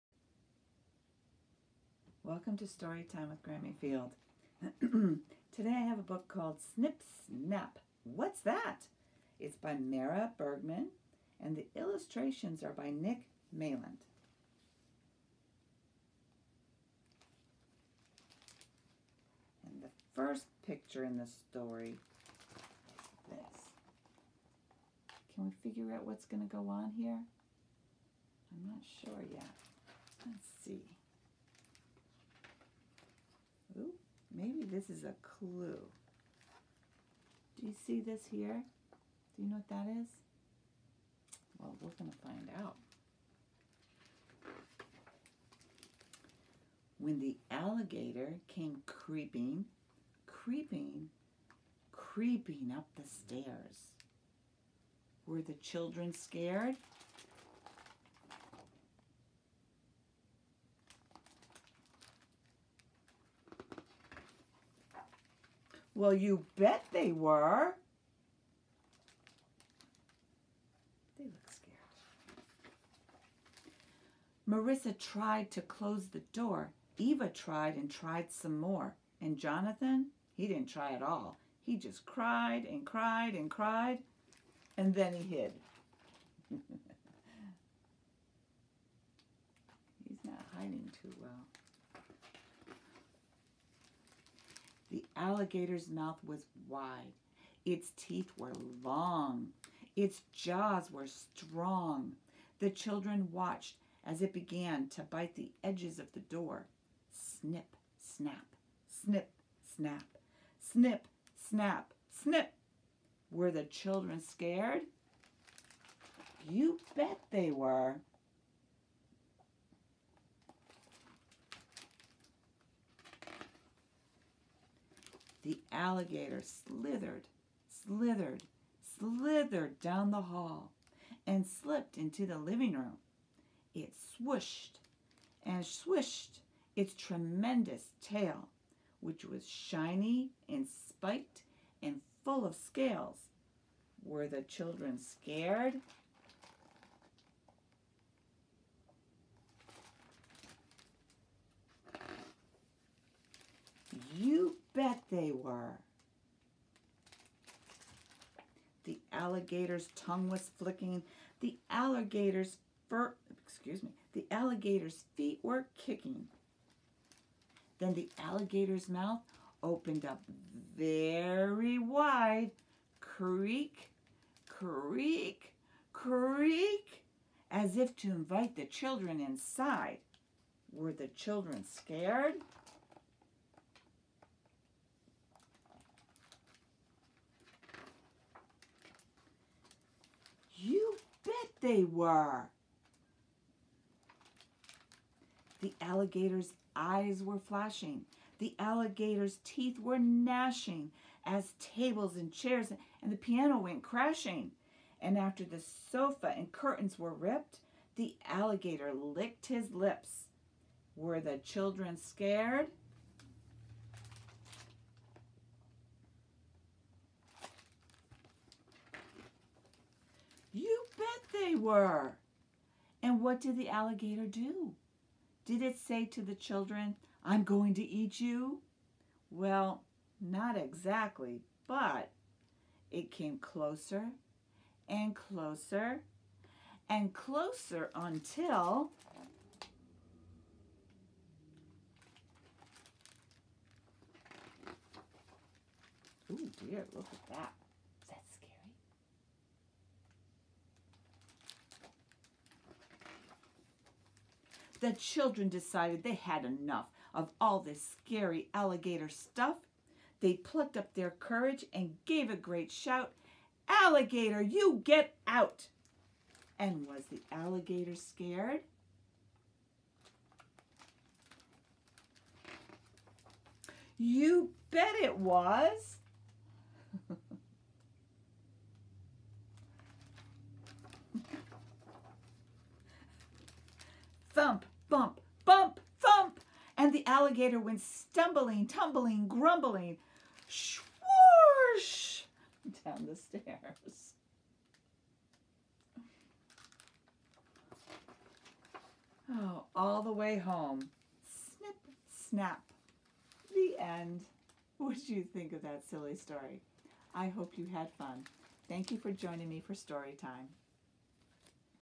Snip Snap What’s That, read by Grammy Field
Snip, Snap, What's That? By Mara Bergman, Illustrations by Nick Maland and read by Grammy Field

The alligator is up to no good. What will the children do?

Have a fun time listening,

love,

Grammy Field